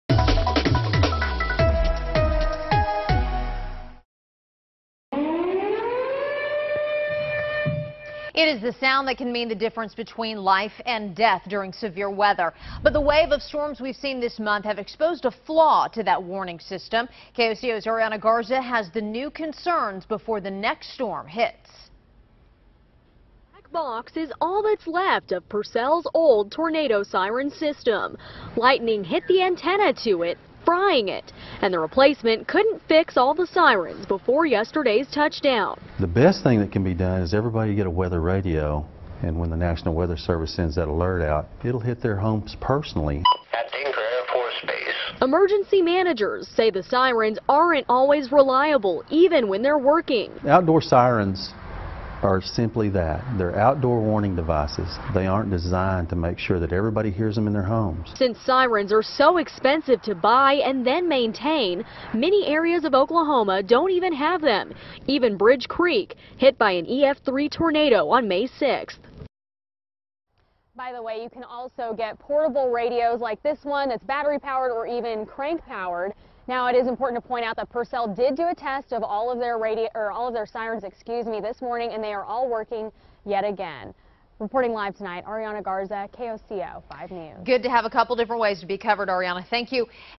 Storms reveal problem with sirens
This week's storms revealed a problem with sirens in Purcell. The city says they have now fixed the issues.